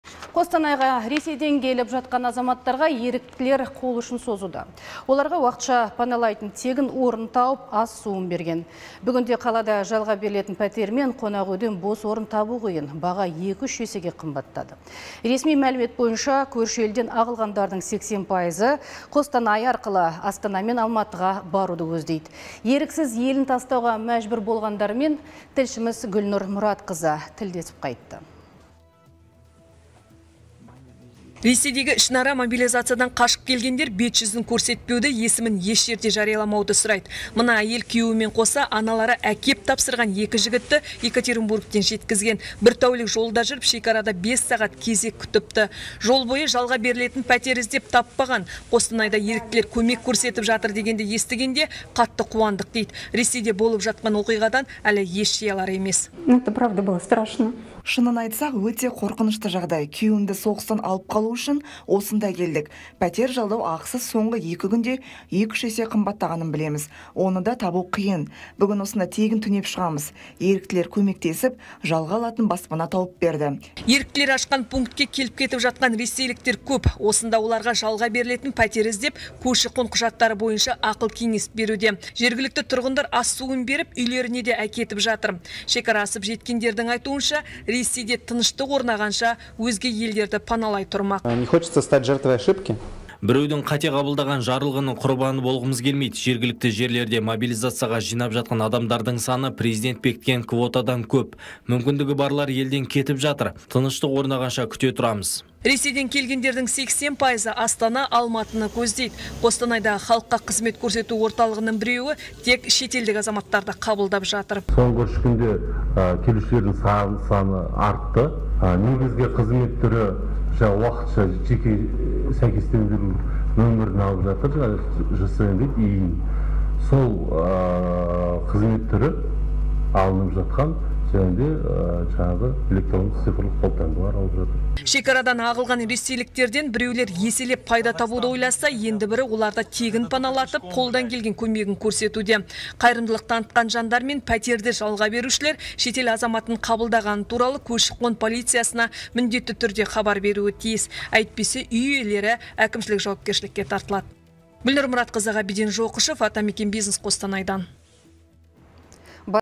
Ресей азаматтарына еріктілер көмек көрсетуде
Қостанайға Ресейден келіп жатқан азаматтарға еріктілер қолұшын созуда. Оларға уақытша  паналайтын тегін орын тауып, ас-суын берген.  Бүгінде қалада жалға берілетін пәтер мен қонақүйден бос орын табу қиын. Баға 2-3 есеге қымбаттады. Ресми мәлімет бойынша көрші елден ағылғандардың 80 пайызы Қостанай арқылы Астана мен Алматыға баруды көздейді. Еріксіз елін тастауға мәжбүр болғандармен тілшіміз Гүлнұр Мұратқызы тілдесіп қайтты.